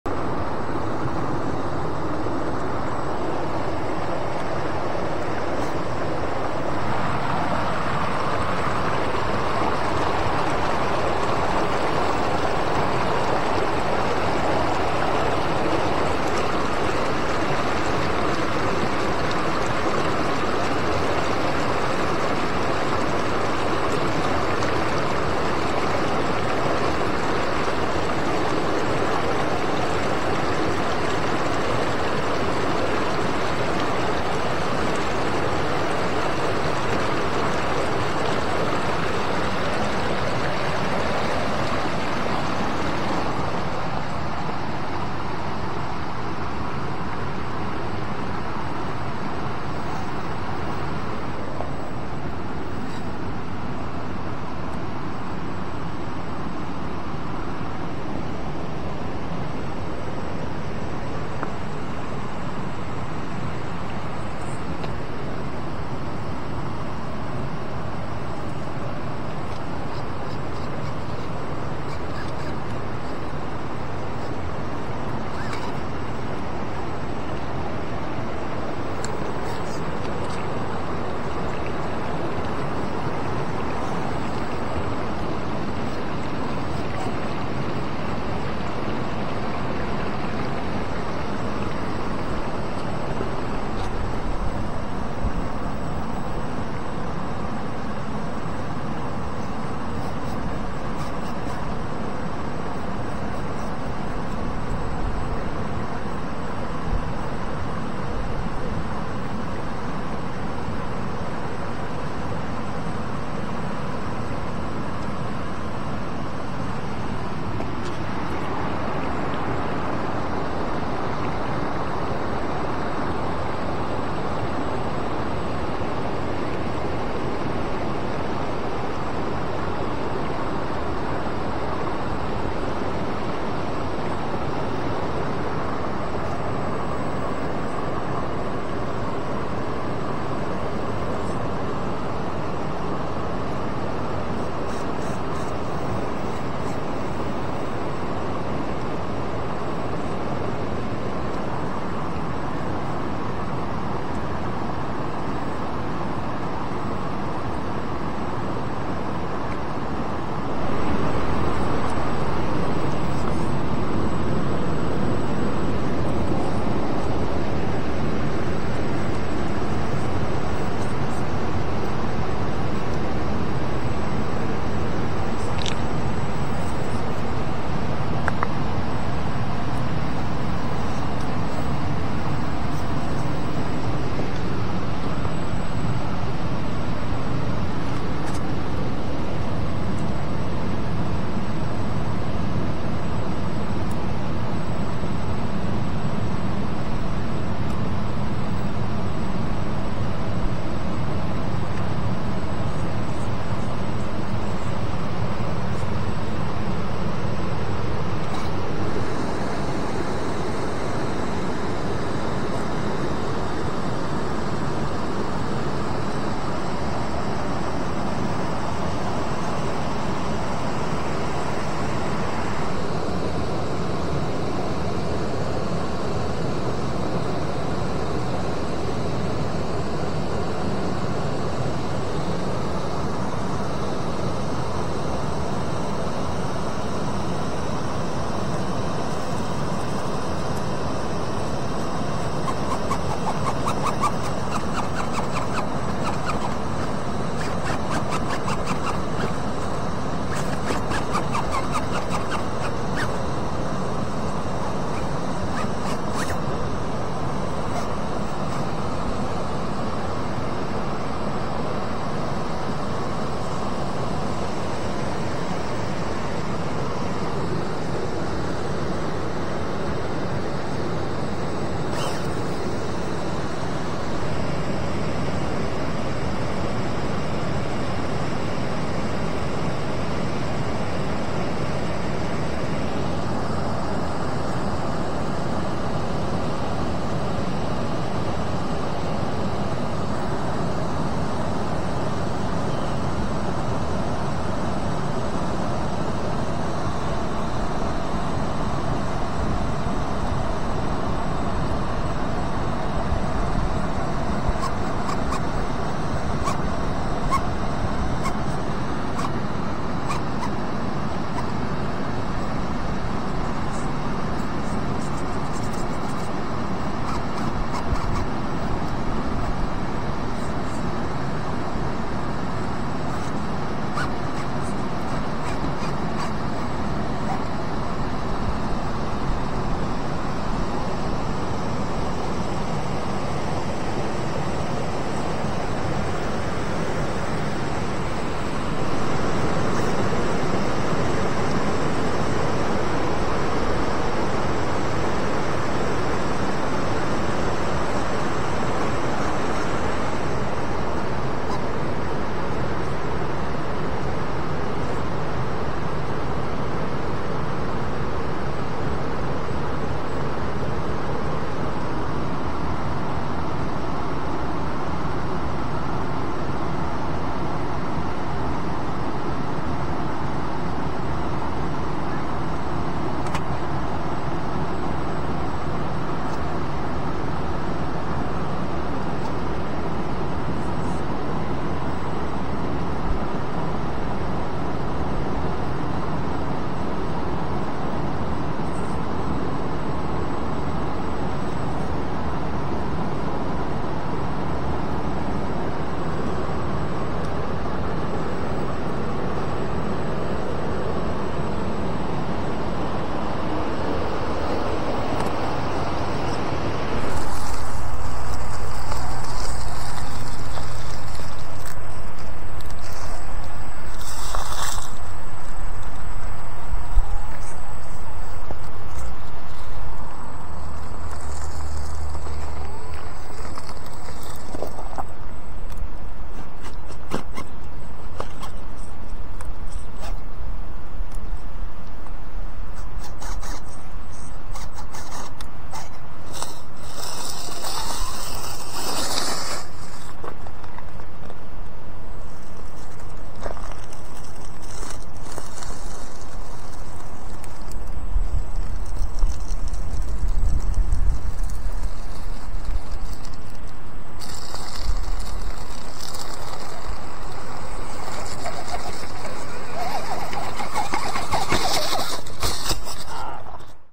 RC4WD Bully II Cascades #3
Part 3 of me learning how to drive the Bully II.

Rig / Parts Breakdown
RC4WD Bully II - Stock RTR - Electronics, Transmitter, Tires, Foams, etc.
Dlux Dravtech 90mm w/Soft Springs, 30w oil on all 4, (1) One Preload ring added on Rear Shocks
No Name 70Kg Brushless Servo from Amazon
RC4WD Bully II Brass Knuckle Weights + 28 grams lead tape on each front, 24 grams on each rear
RC4WD Bully II Upgrade Steel U-Joint Shafts
RC4WD Angled Rod Ends on F/R Upper Links
Wheelbase Shortened to 12 3/8"
C-Hubs Clocked to 35 Degrees and Rear Axle Approx 30 Degrees
Custom Sheet Metal Roof made by Yours Truly